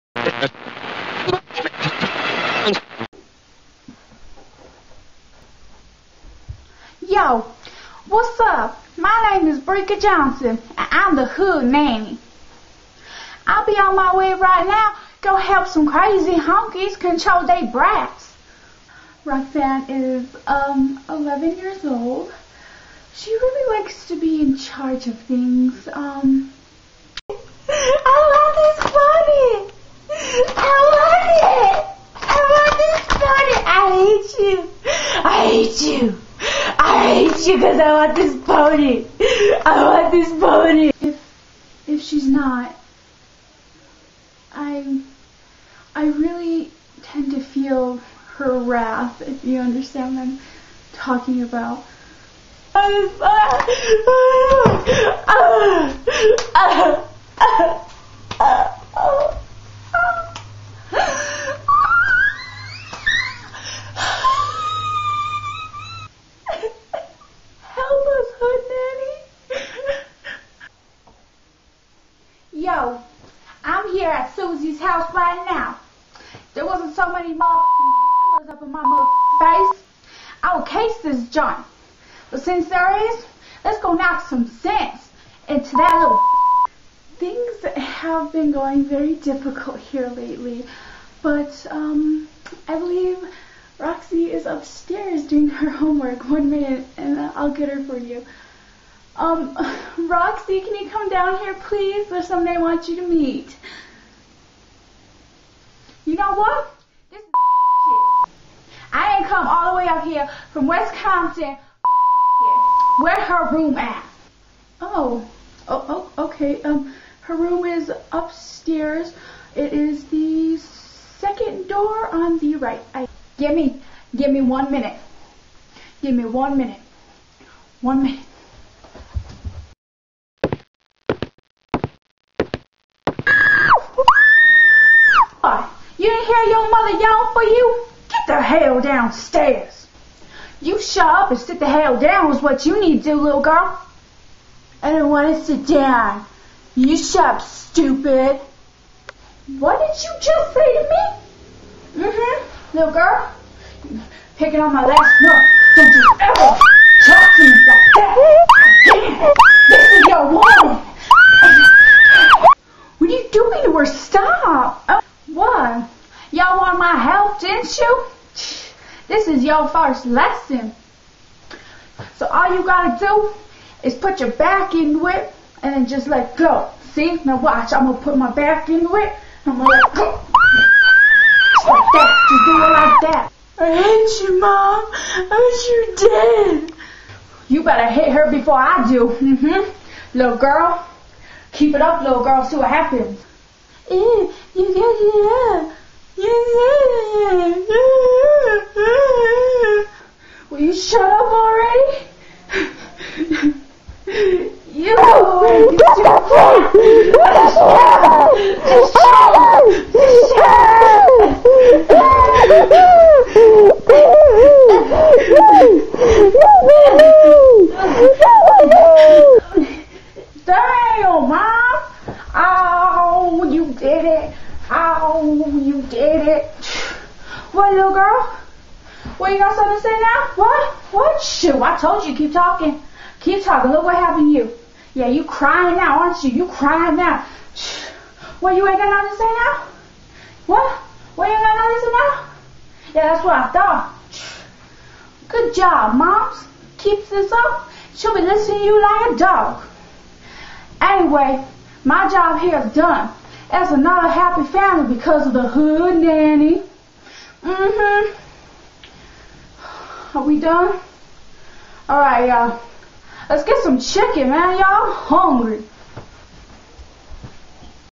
Hood Nanny 911
This out of control Nanny from the Hood helps this Mother learn better parenting skills by literally BEATING some manners into her 11yr old daughter Roxanne!
WARNING: NO CHILDREN WHERE HARMED DURING THE MAKING OF THIS FILM!!!